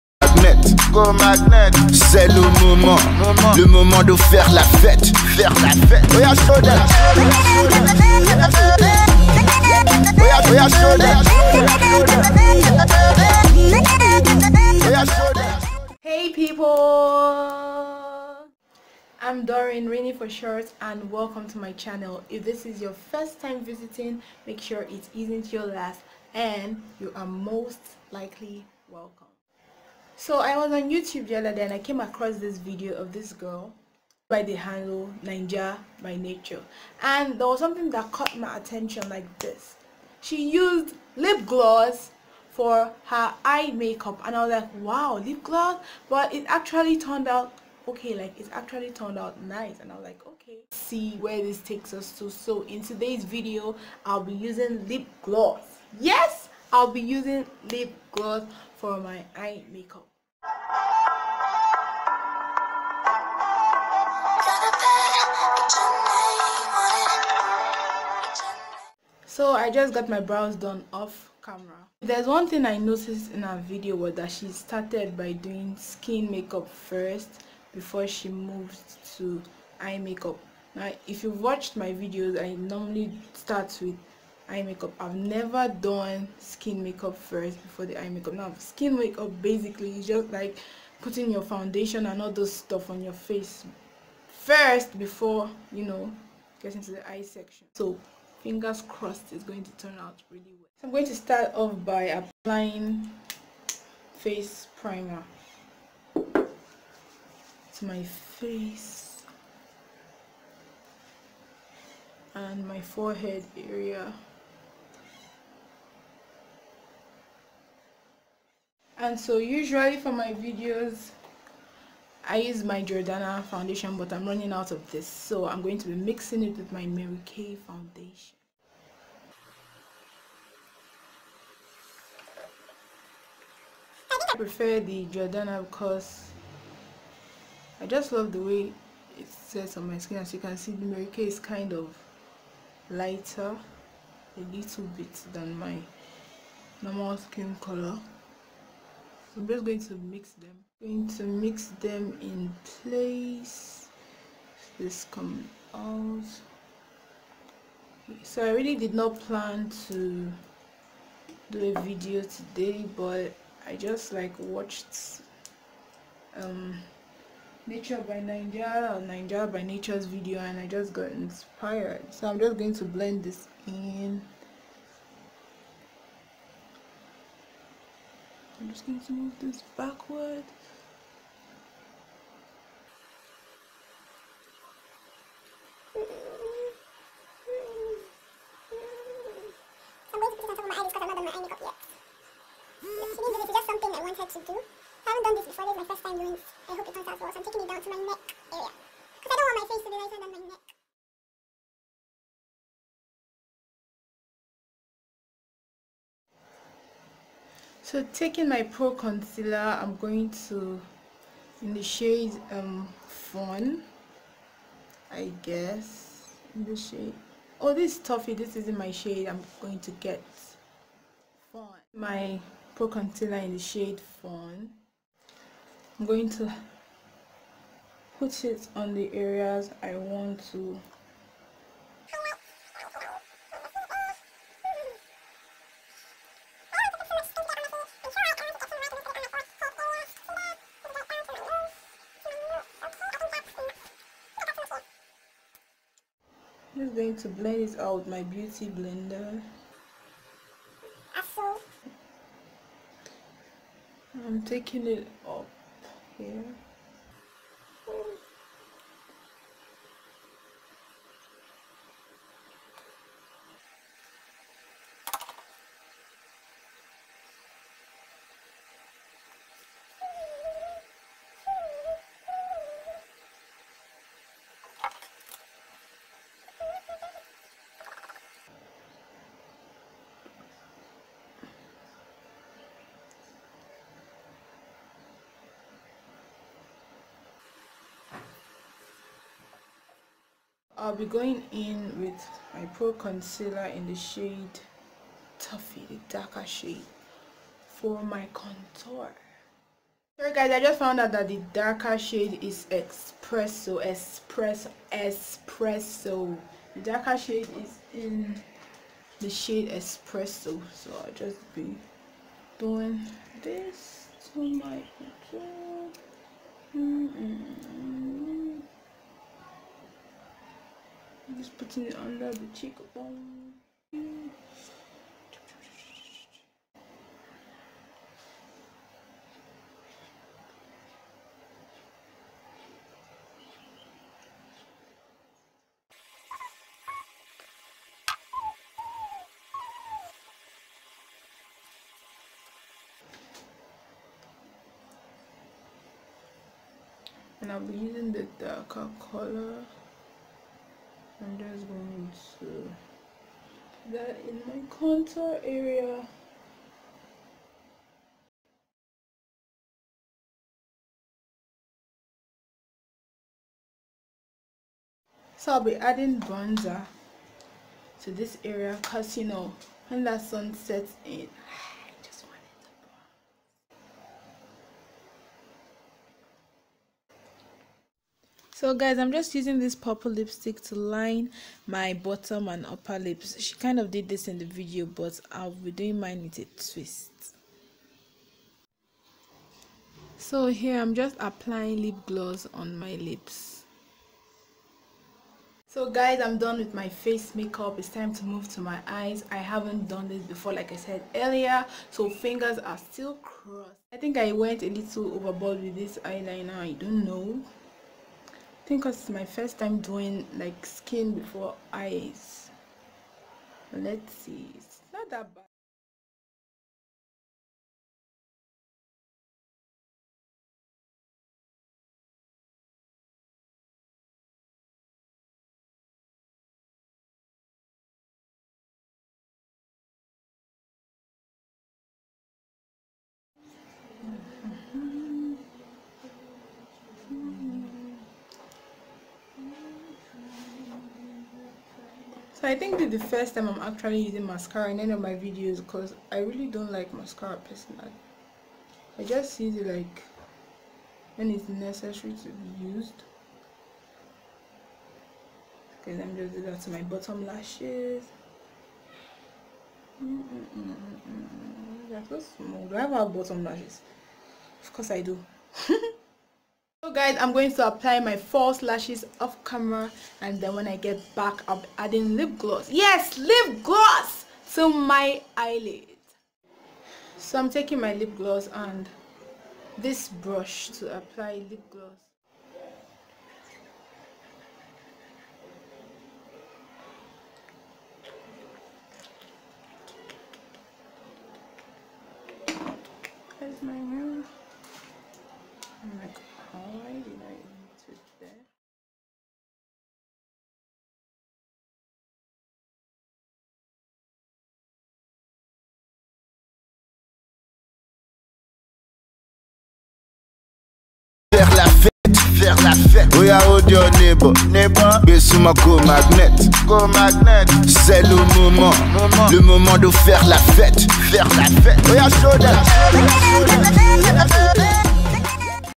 LIP GLOSS EYELIDS + MATTE FACE: INSPIRED BY NAIJAABYNATURE
I was on youtube the other day and came accross naijaabynature 's videos and I was surprised yet impressed at how she substituted lipgloss for eyeshadow. I hops I did great.